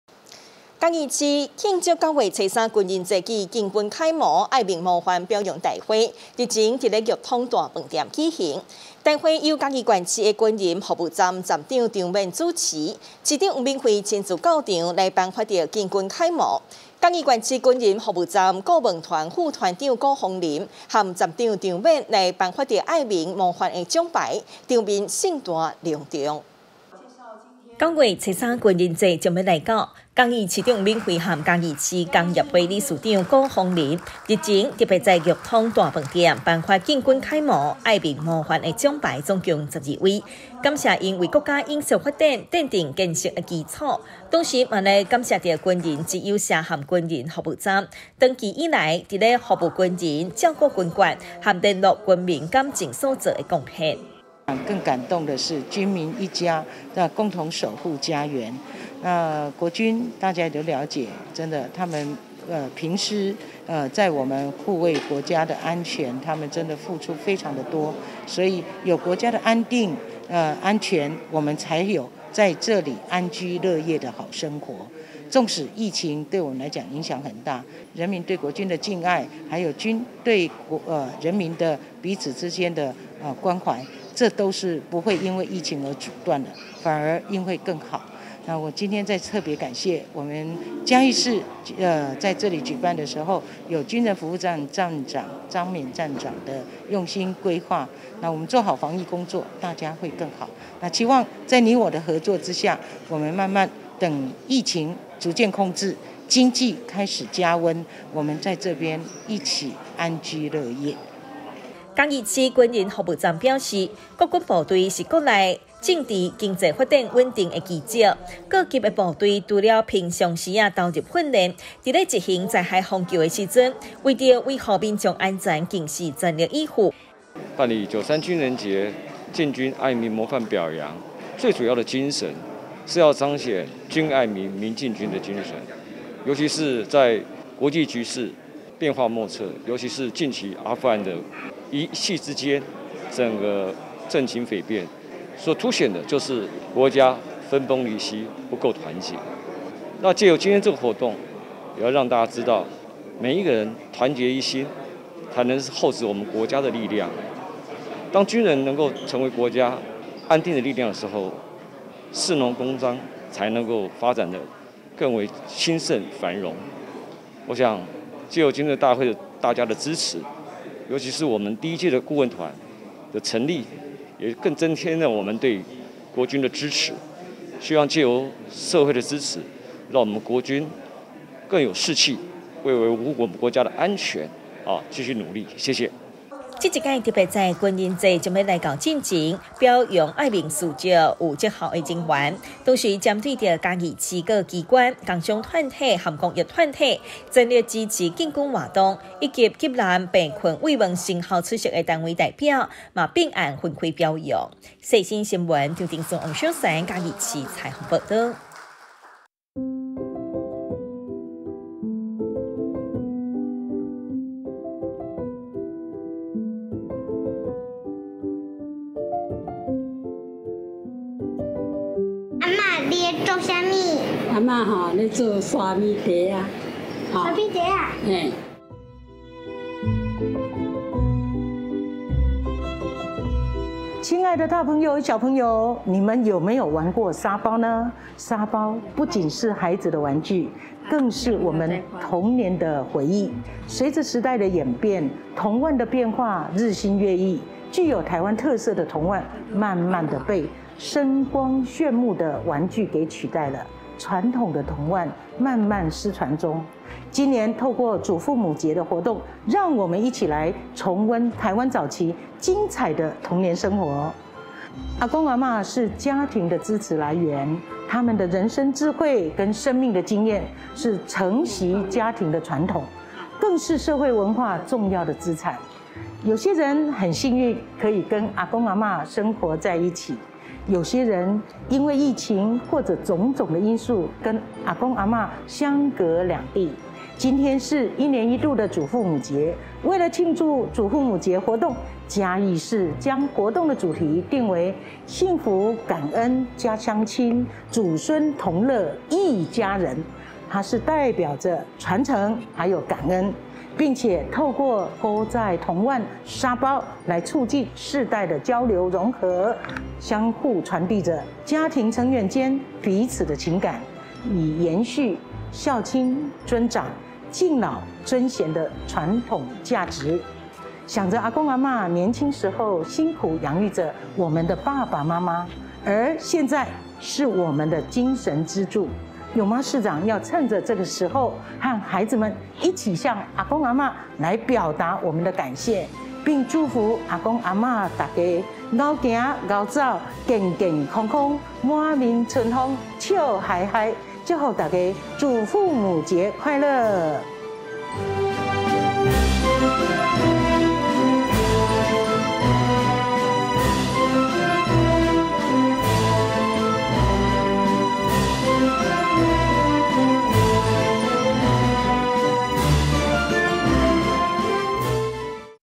世新新聞 慶祝93軍人節 嘉義市表揚敬軍楷模、愛民模範
110年08月28日 (六) 世新新聞

慶祝93軍人節 嘉義市表揚敬軍楷模、愛民模範

嘉義市慶祝九三軍人節暨敬軍楷模、愛民模範表揚大會，日前在鈺通大飯店舉行，大會由嘉義縣市軍人服務站站長張勉主持，嘉義市長黃敏惠親自到場，頒發敬軍楷模，嘉義縣市軍人服務站顧問團副團長古宏麟與站長張勉，頒發愛民模範獎牌，場面盛大隆重。